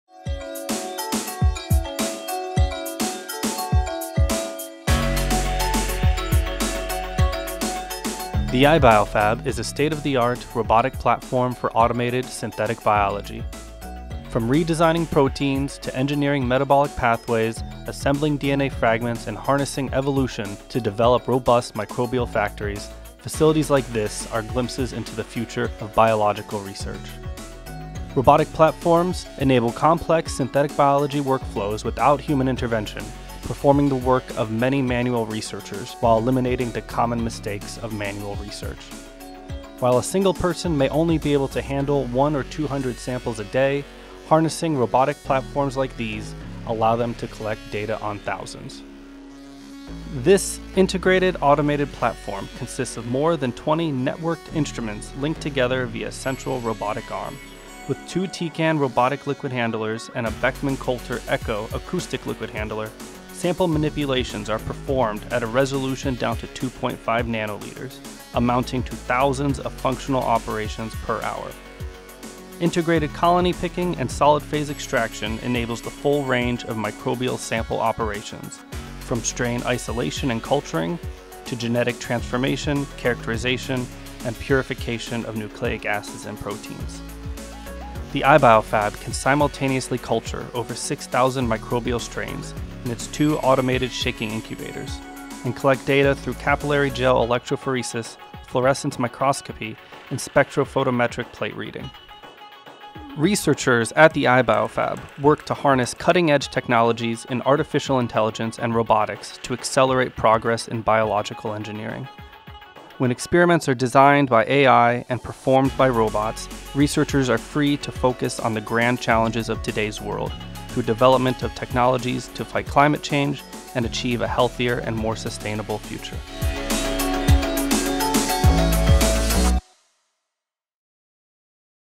The Illinois Biological Foundry for Advanced Biomanufacturing (iBioFAB)
This video is designed to show off just some of the capabilities of the iBioFAB, housed inside the Carl R. Woese Institute for Genomic Biology on the University of Illinois campus.  The iBioFAB aims to accelerate the biological engineering process by integrating artificial intelligence/machine learning with automation. This synthetic biology foundry features a 6-degree-freedom articulated robotic arm that travels along a 5-meter-long track to transfer microplates among more than 20 instruments installed on the platform.

The system houses three liquid handling devices: the nanoliter-scale Labcyte Echo 550 acoustic liquid handler in addition to the pipet-based Tecan Freedom EVO 200 and Tecan Fluent 1080 systems.
At maximum capacity, this biofoundry can house over 500 96-well plates within its four Thermo-Fisher Cytomat automated incubators. Coupled with an integrated laboratory information management system (LIMS) and scheduling software, the iBioFAB can generate thousands of output samples each day using custom-designed workflows for plasmid assembly, microbial transformations, and more.